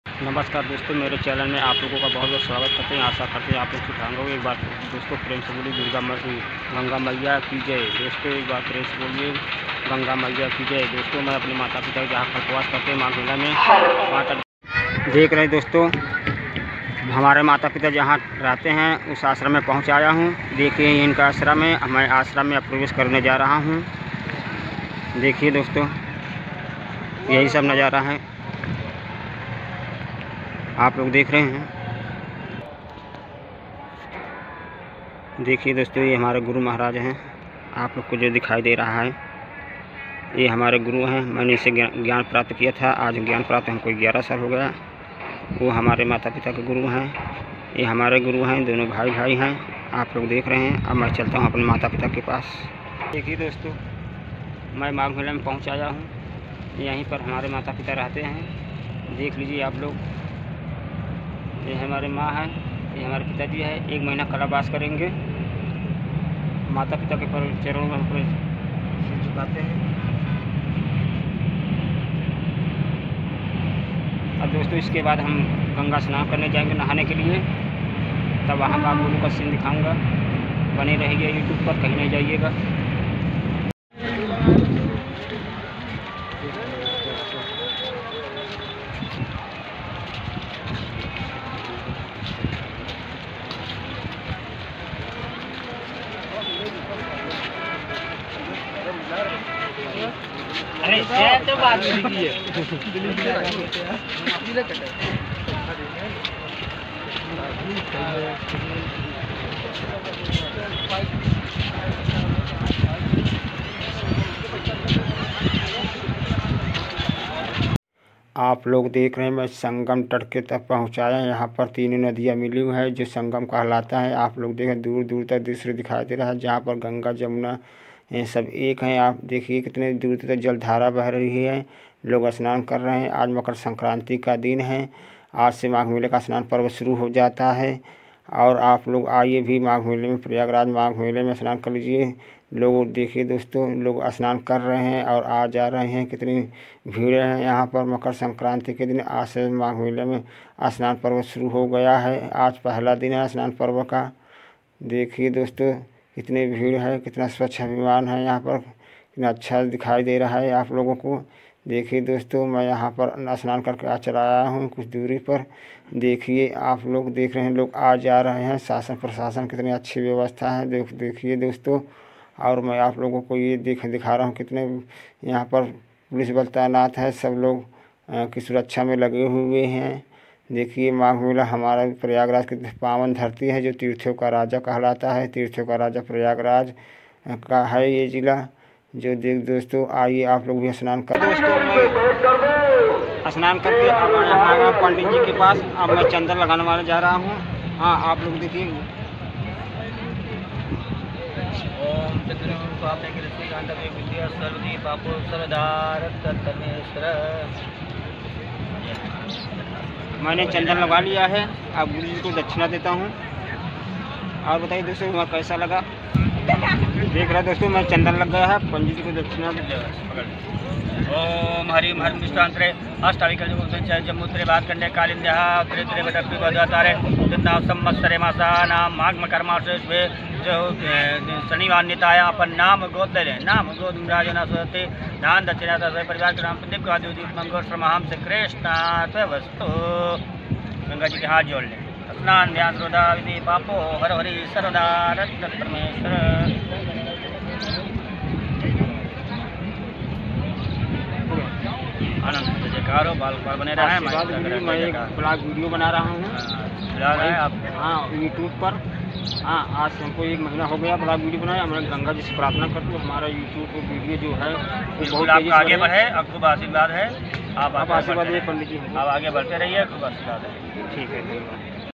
14जनवरी आज मकर संक्रांति है|माघ मेला में स्नान का पहला दिन है|ramachalpatel7
14जनवरी आज मकर संक्रांति है |माघ मेला में स्नान का पहला दिन है|ramachalpatel7#dailysvilog#youtube